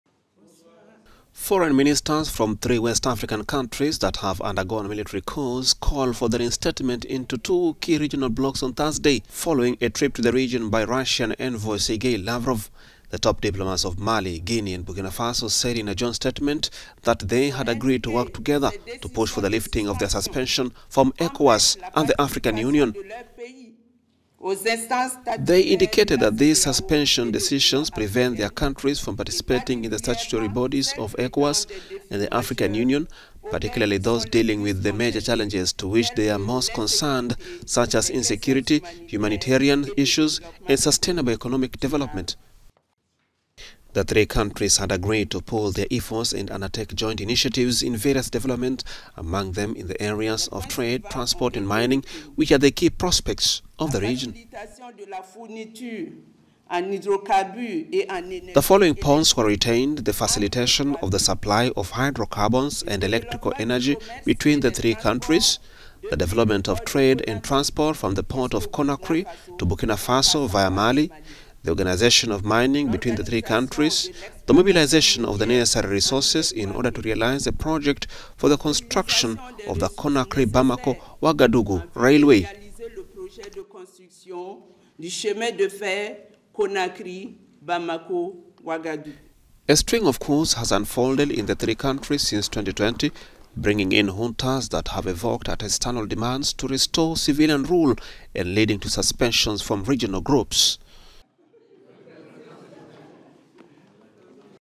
West Africa junta regimes seek reentry to regional blocs
Foreign ministers from three West African countries that have undergone military coups called for their reinstatement into two key regional blocs on Thursday following a trip to the region by Russian envoy Sergei Lavrov.